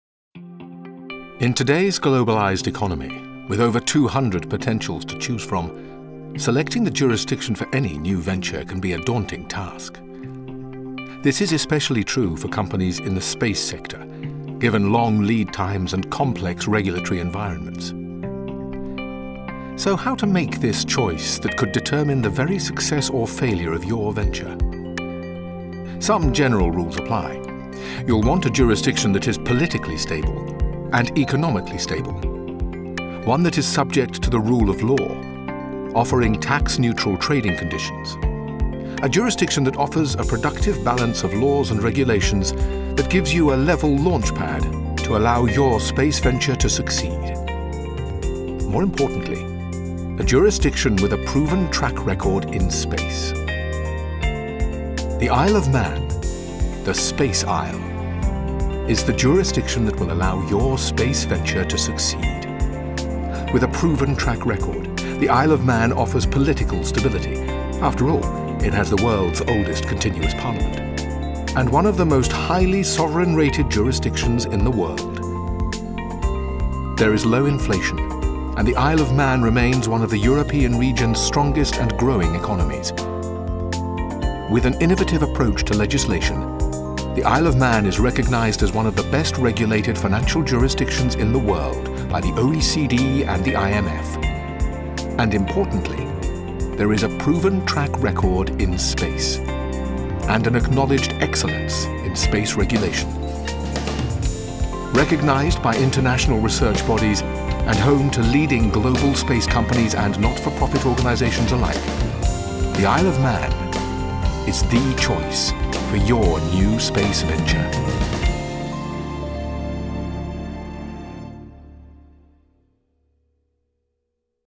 Isle of Man - The launch pad your space-related business needs to succeed.
Find out why the Isle of Man is increasingly being recognised as a leading location for space-related businesses.